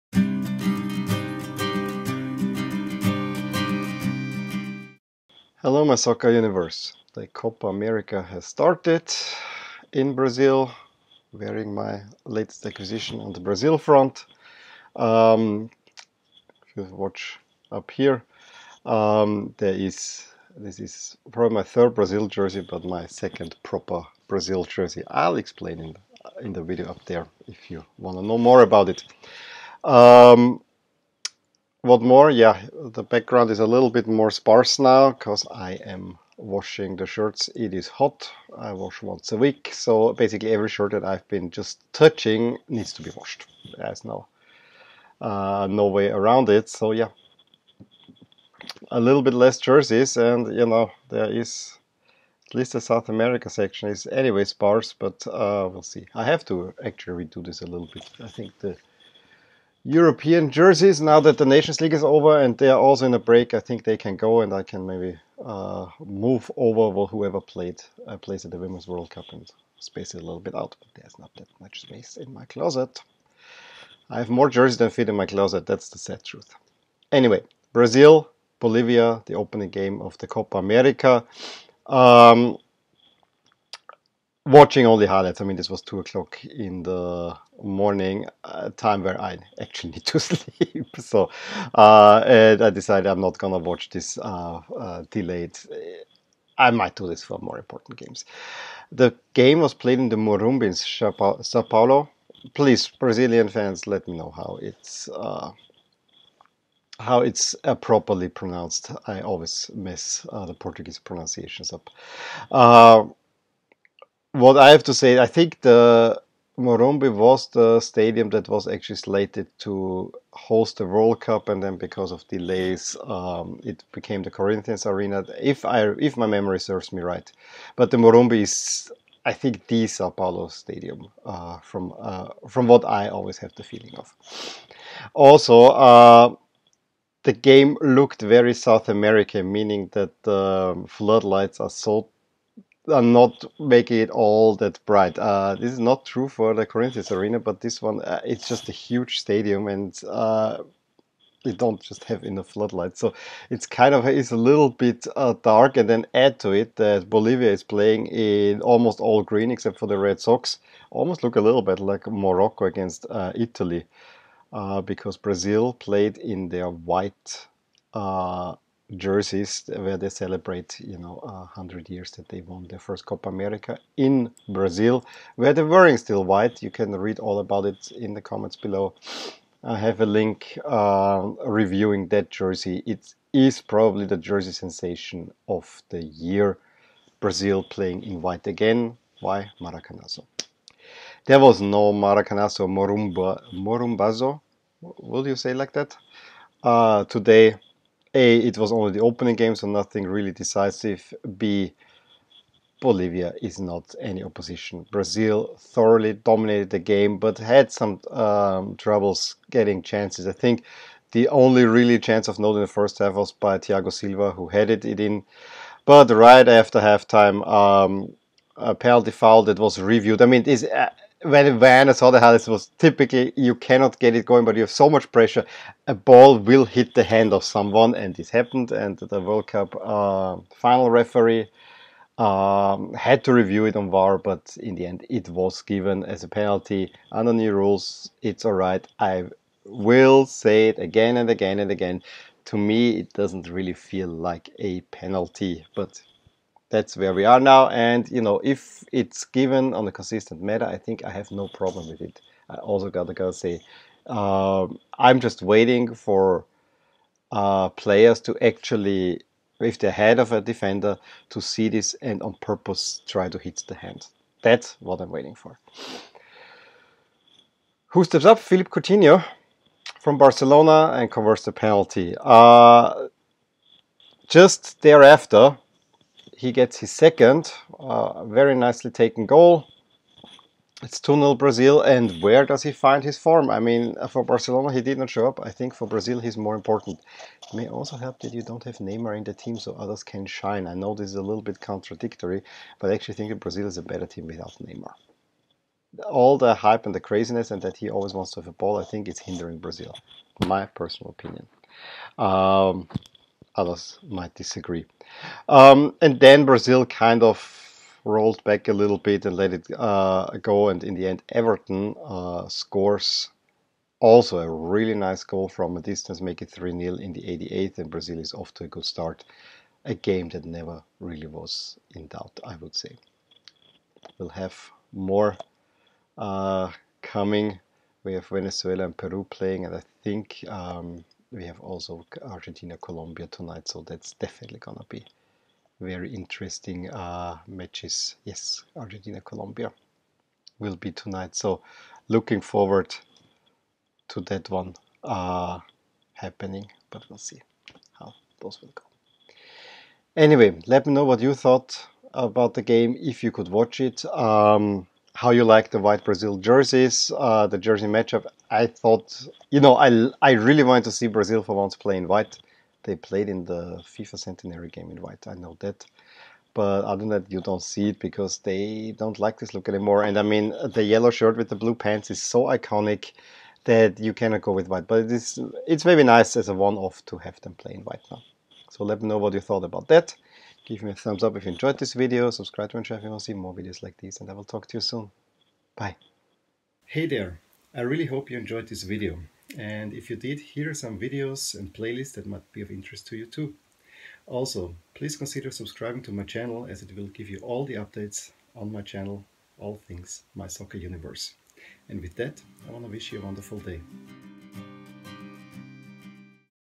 Brazil With a Solid Start to the Copa America
A remarkable jersey match up sees Coutinho step up and get the Selecao off to a good start.